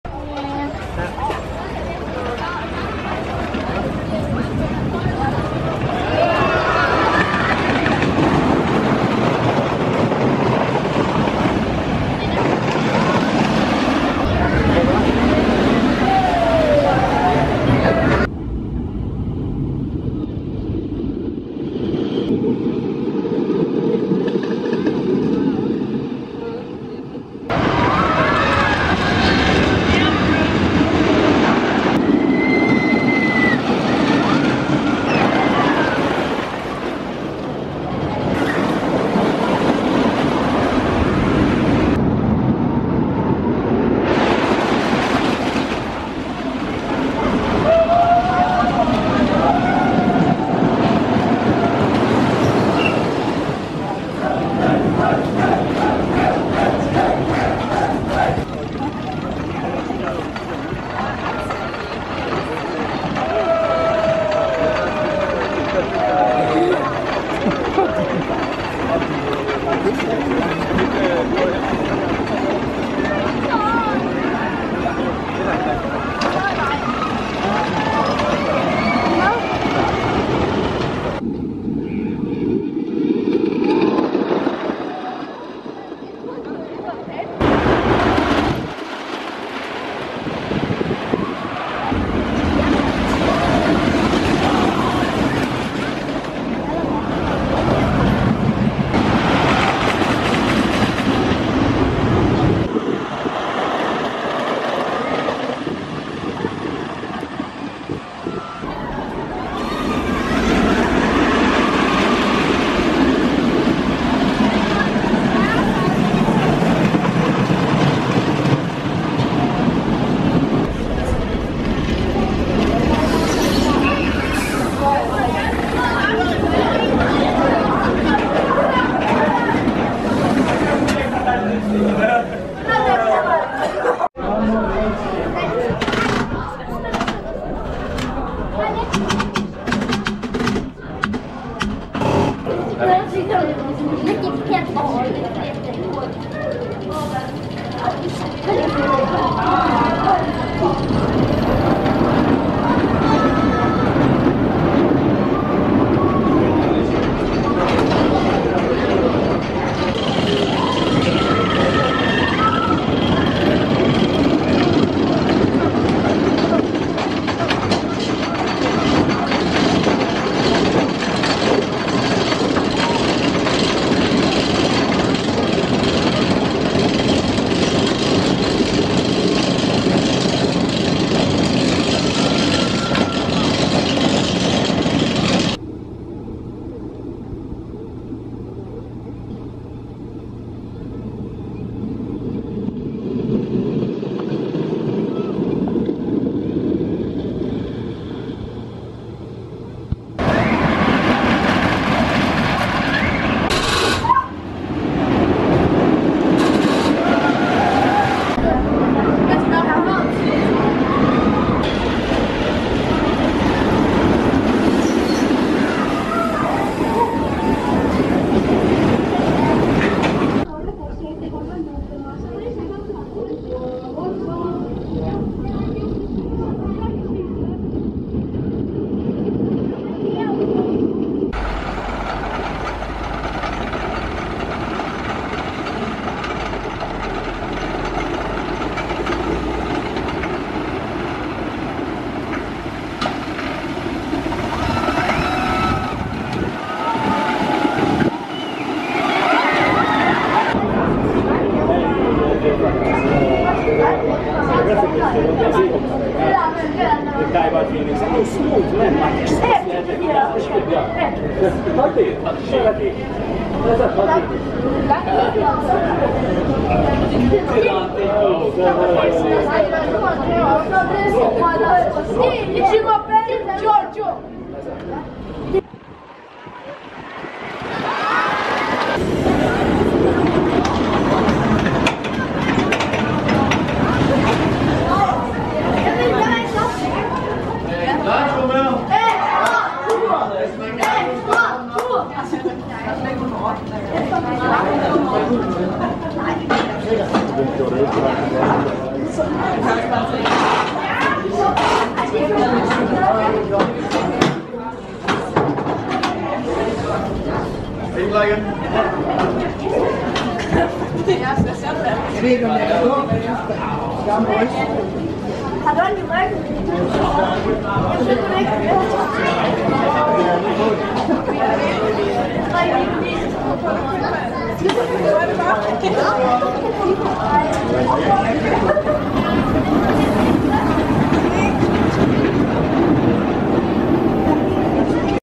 Falken Off-Ride Footage, Farup Sommerland S&S Wood Coaster | Non-Copyright
Falken at Farup Sommerland is one of the few wood coasters in Denmark.  This ride was designed by Alan Schilke and it is one of four wood coasters built by Alan Schilke.  The ride has a reputation for roughness, but it does also have some sweet airtime.